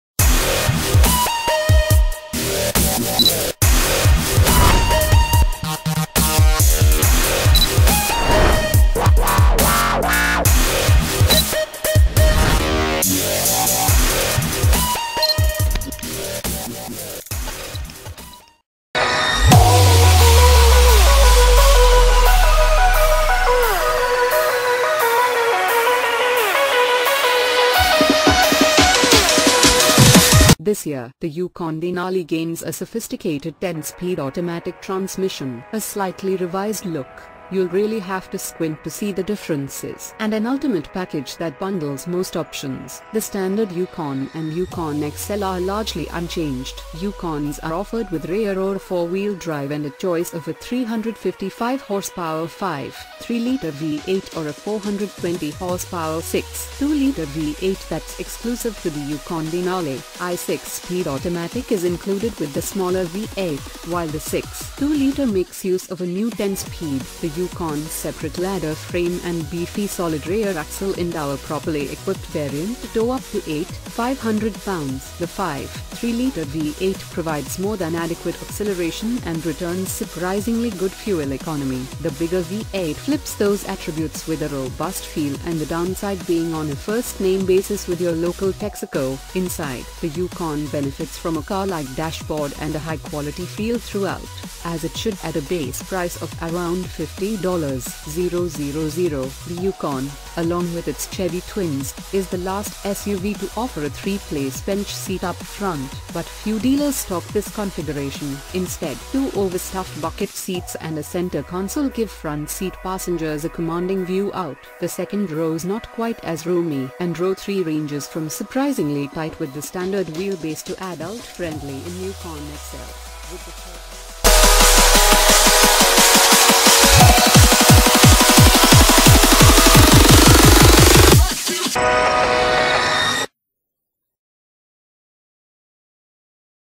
WOW AMAZING!! 2018 GMC Yukon Spec & Price
WOW AMAZING!! 2018 GMC Yukon Spec & Price - This year, the Yukon Denali gains a sophisticated 10-speed automatic transmission, a slightly revised look (you’ll really have to squint to see the differences), and an Ultimate Package that bundles most options. The standard Yukon and Yukon XL are largely unchanged.

$49,080

Popular Post !